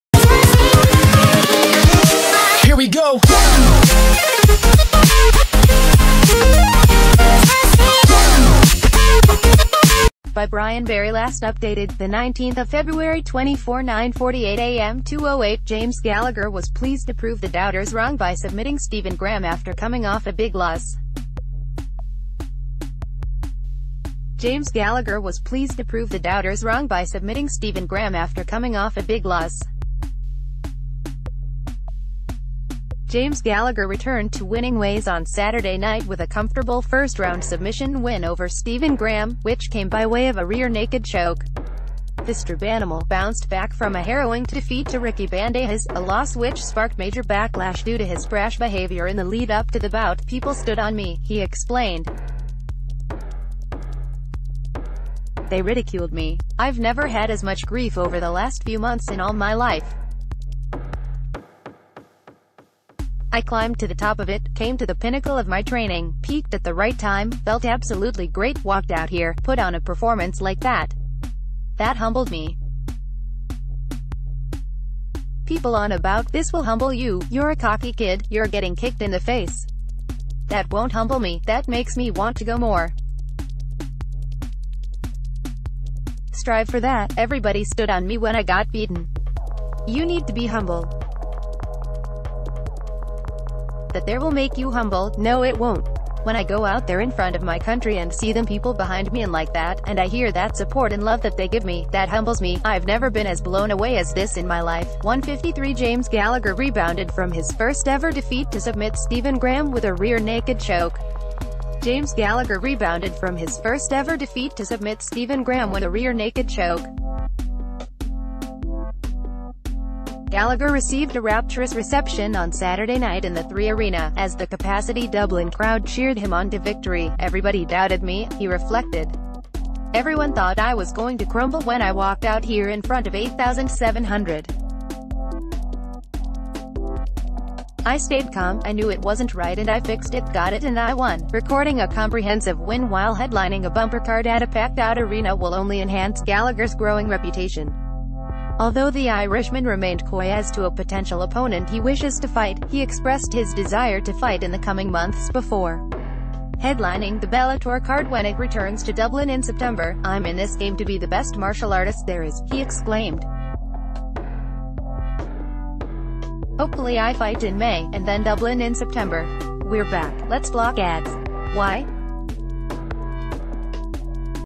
Video: James Gallagher thrilled to silence doubters
✔ By Brian Barry Last Updated: 24/02/19 9:48am 2:08 James Gallagher was pleased to prove the doubters wrong by submitting Steven Graham after coming off a big loss. James Gallagher was pleased to prove the doubters wrong by submitting Steven Graham after coming off a big loss. James Gallagher returned to winning ways on Saturday night with a comfortable first-round submission win over Steven Graham, which came by way of a rear naked choke. 'The Str...